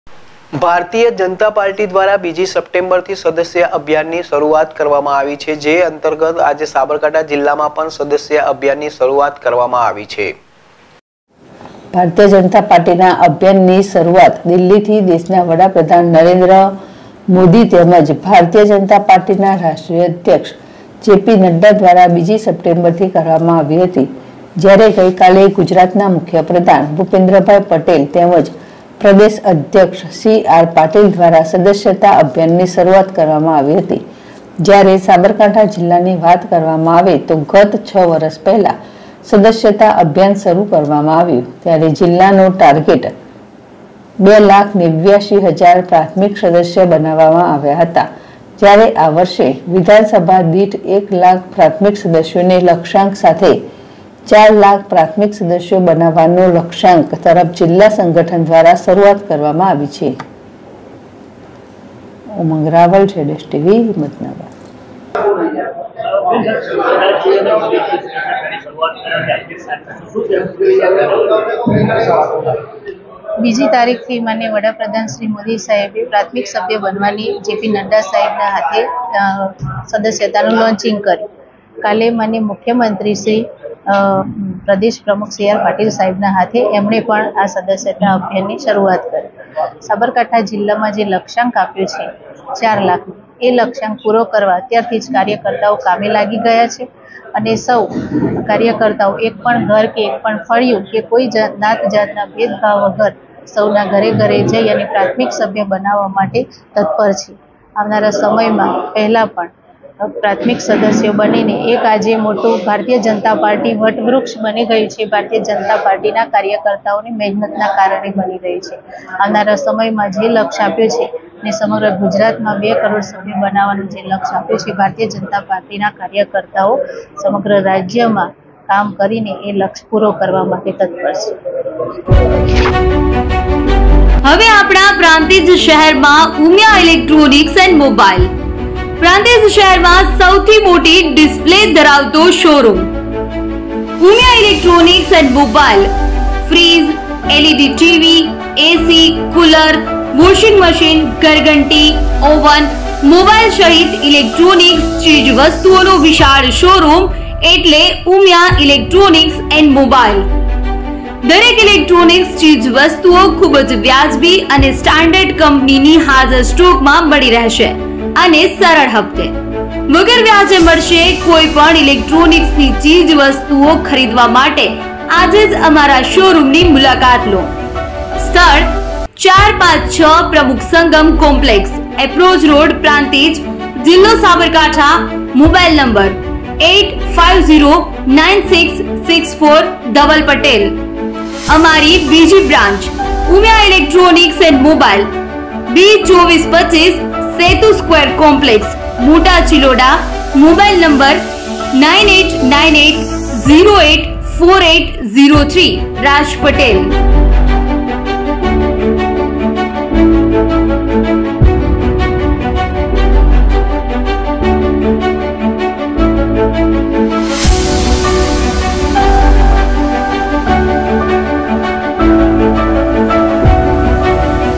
ભારતીય જનતા પાર્ટી દ્વારા બીજી સપ્ટેમ્બરથી સદસ્યતા અભિયાનની શરૂઆત કરવામાં આવી છે
સાબરકાંઠા ભારતીય જનતા પાર્ટી દ્વારા બીજી સપ્ટેમ્બરથી સદસ્યતા અભિયાનની શરૂઆત કરવામાં આવી છે